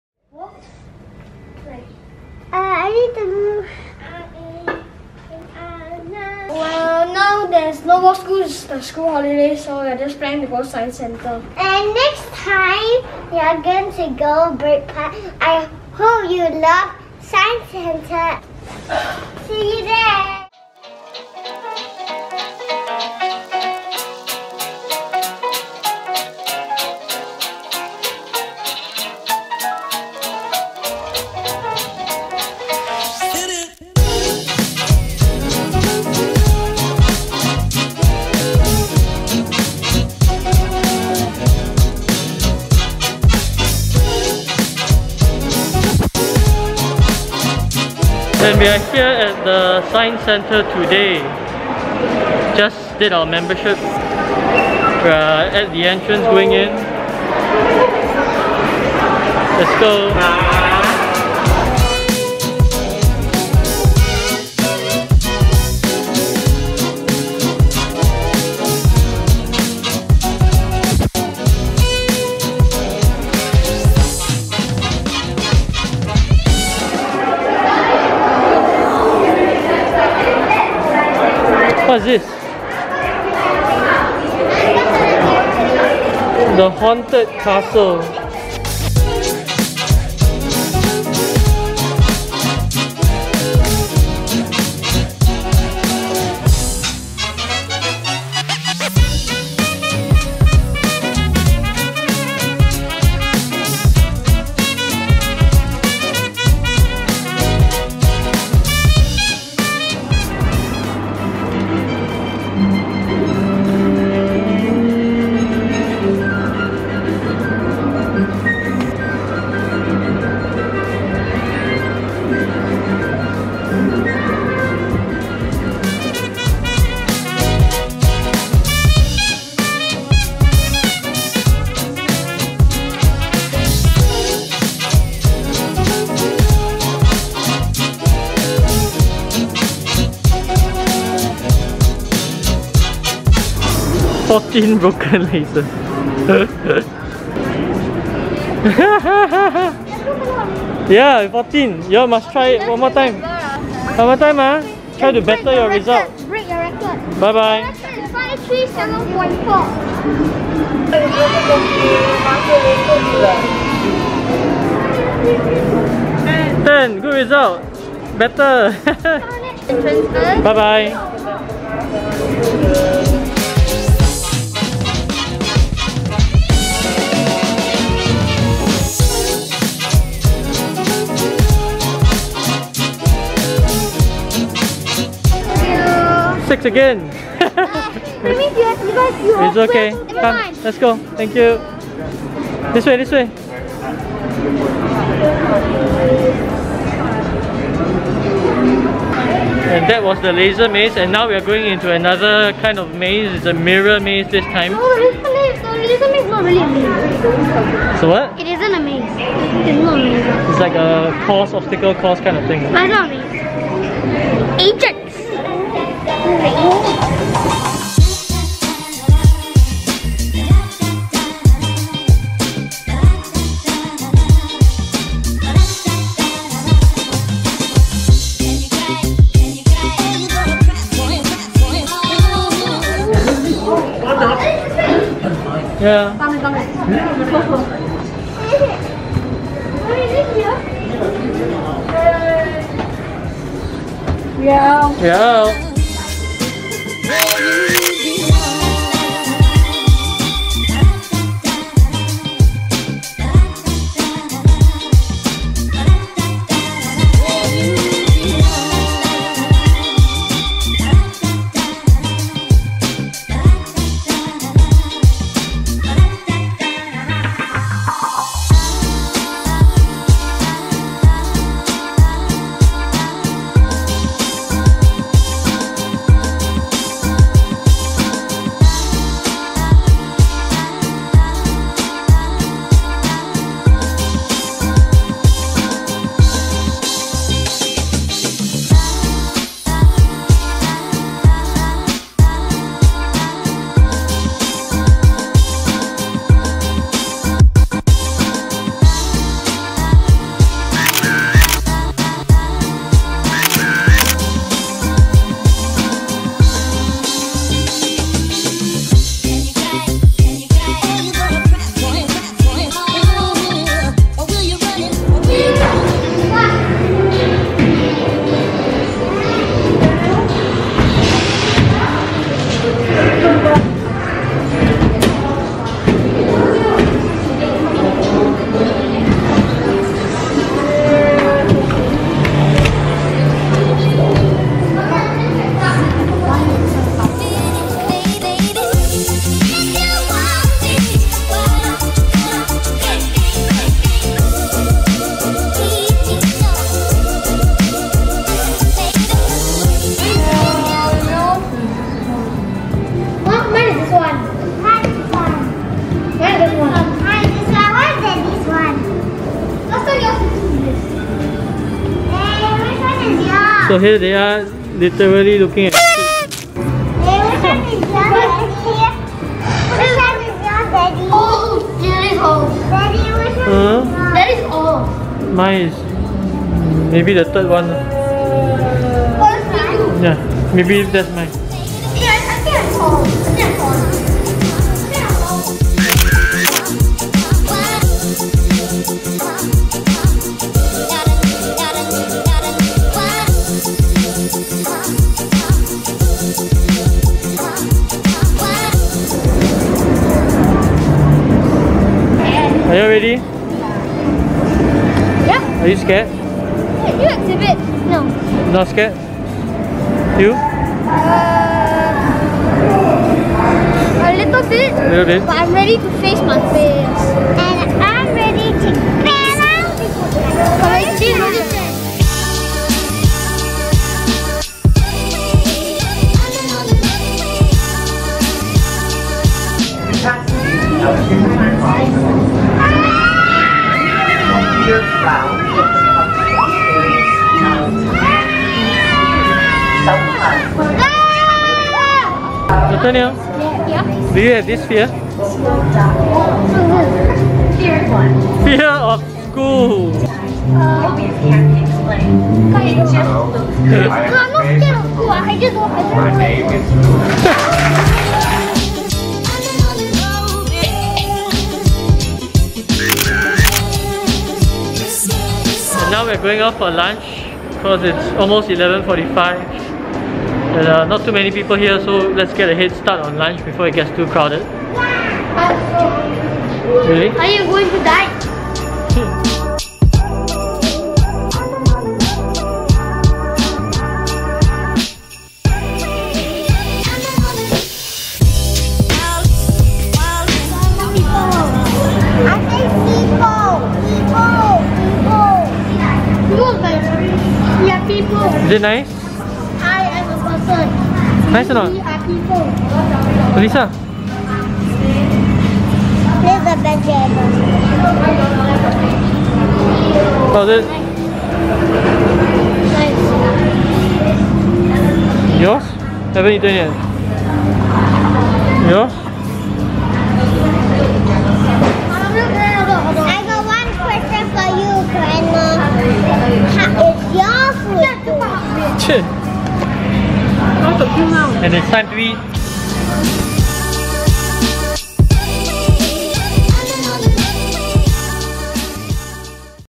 LEARNING ABOUT POO & FARTS @ THE SCIENCE CENTRE! | EP:153
We renewed our annual membership and went to most of our favourite exhibits. Take note though that hours are limited because its closed from 1-2pm for cleaning.

Welcome to our family channel! We're the Lau family, and this is probably the first weekly family vlog from Singapore!